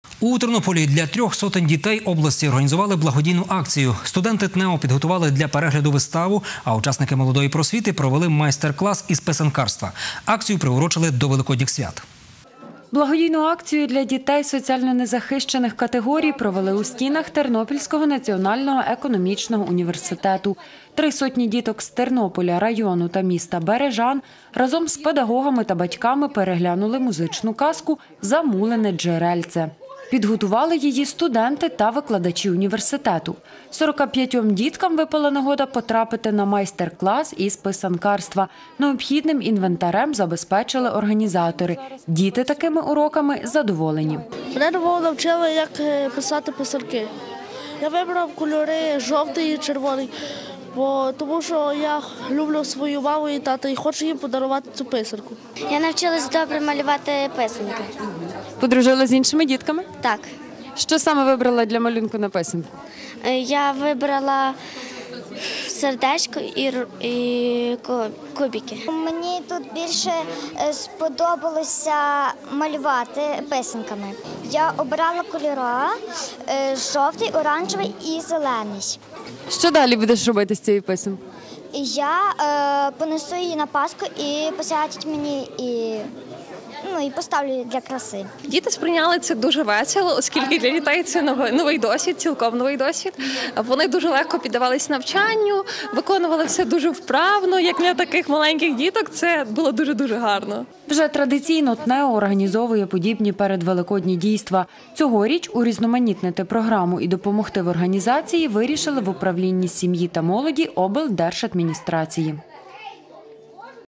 У Тернополі для трьох сотень дітей організували благодійну акцію
У Тернополі для трьох сотень дітей області організували благодійну акцію.  Студенти ТНЕУ підготували для перегляду виставу, а учасники «Молодої Проствіти» проводили майстер-клас із писанкарства. Акцію приурочили до Великодніх свят.